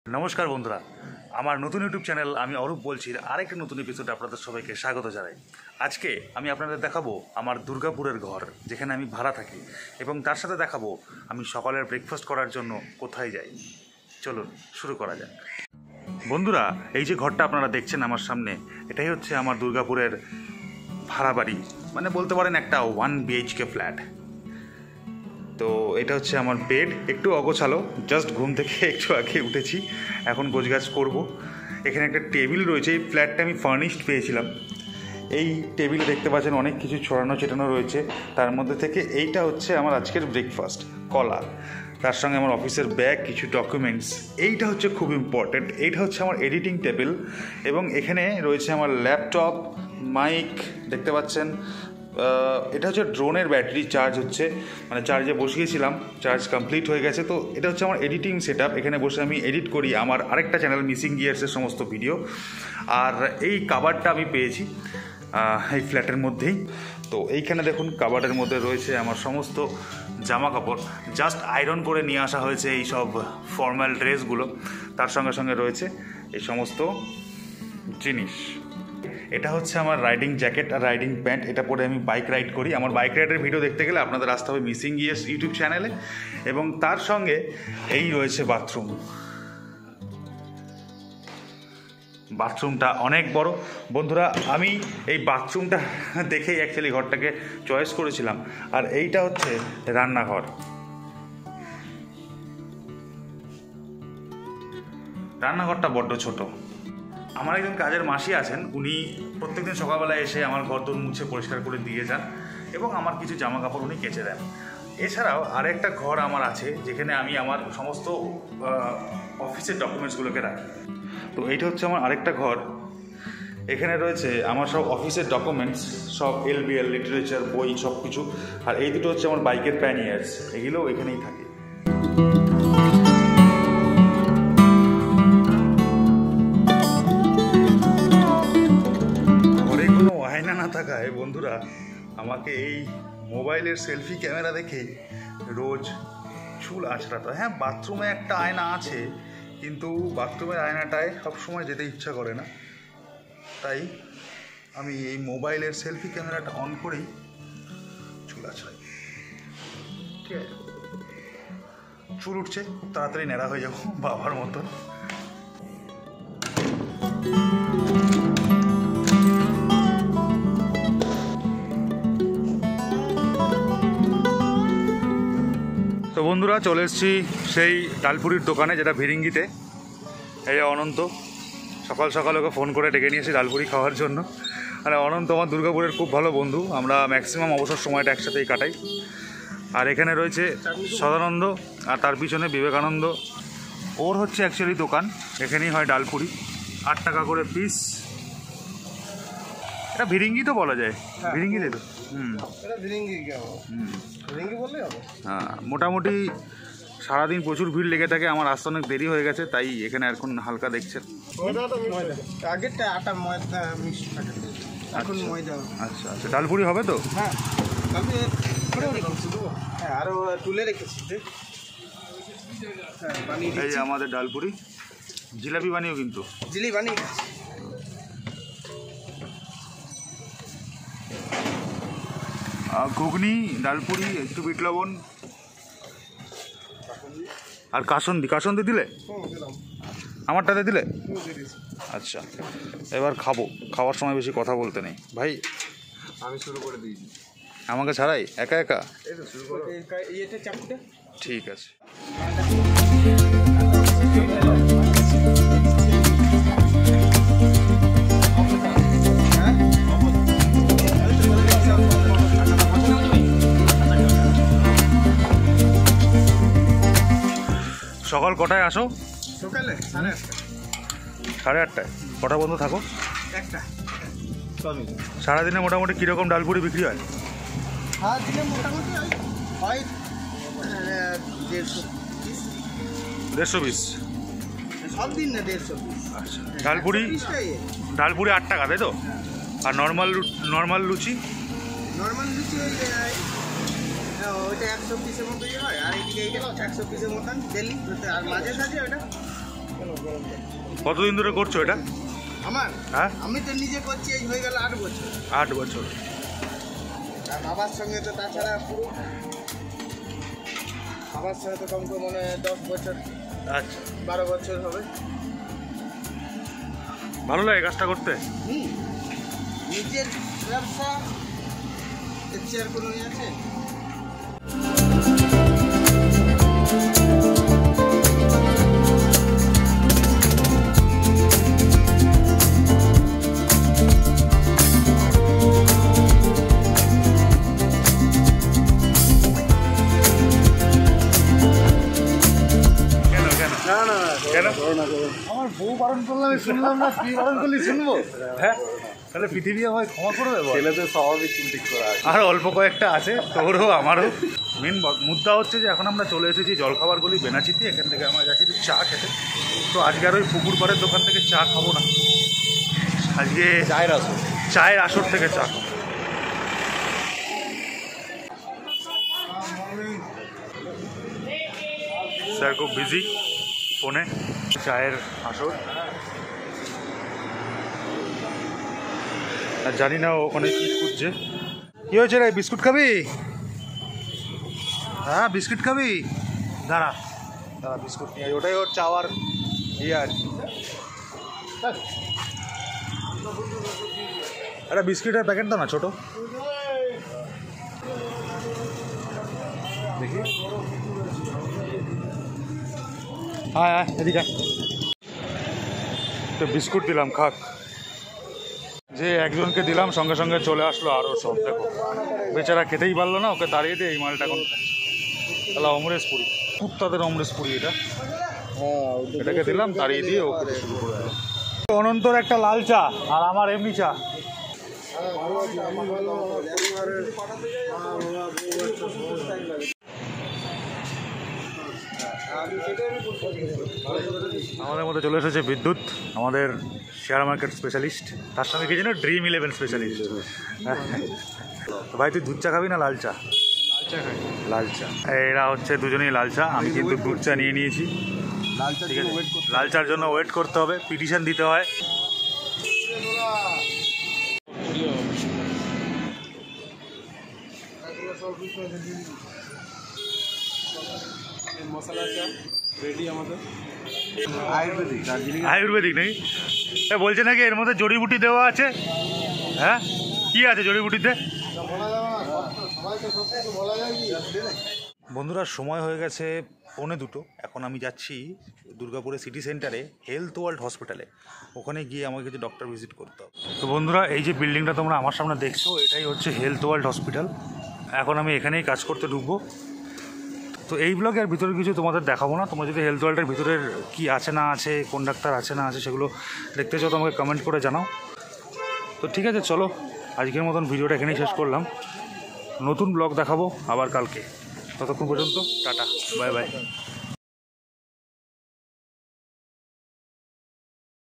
দুর্গাপুরের এই ডালপুরি খেয়েছেন কখনও ? এই ঘরে কিভাবে থাকি আমি 🏠 | কুকুরদের অমরেশ পুরী 😂
দুর্গাপুরের সেরা ডালপুরি | এই ঘরে কিভাবে থাকি আমি 🏠 | মশলা চা নাকি দুধ চা | কুকুরদের অমরেশ পুরী 😂